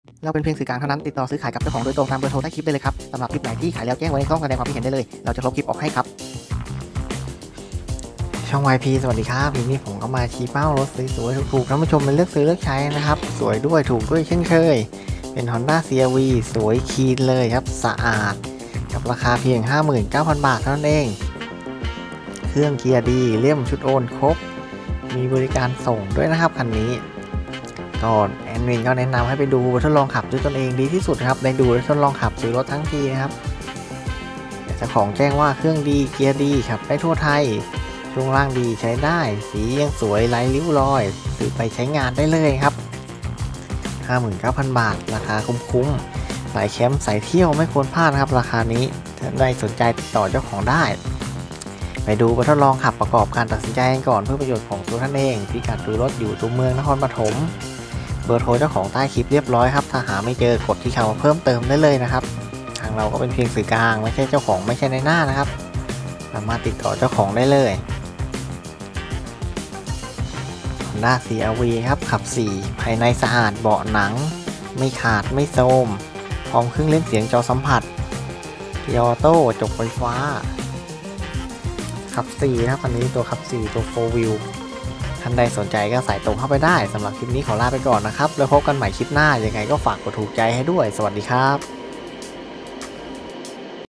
CRV ปี 2000 ‼️ขายด่วน 59,000 บาท👍 ขับ 4×4
Honda CRV ขับ 4x4 เกียร์ออโต้ Vาย💸59000💸เล่มพร้อมโอนภาษี 66 มีบริการส่ง (ส่งฟรี 100 กิโลแรก) ,เครื่องดี เกียร์ดี ขับได้ทั่วไทย ,ช่วงล่างดีใช้ได้ ,สีสวยไร้สิว ,ซื้อไว้ใช้งาน ดีกว่าเดิน 🚗รถอยู่นครปฐมตัวเมือง 👉ละเอียดอย่างอื่นเน้นโทร 📞 0️⃣9️⃣3️⃣6️⃣4️⃣6️⃣9️⃣3️⃣3️⃣3️⃣ พล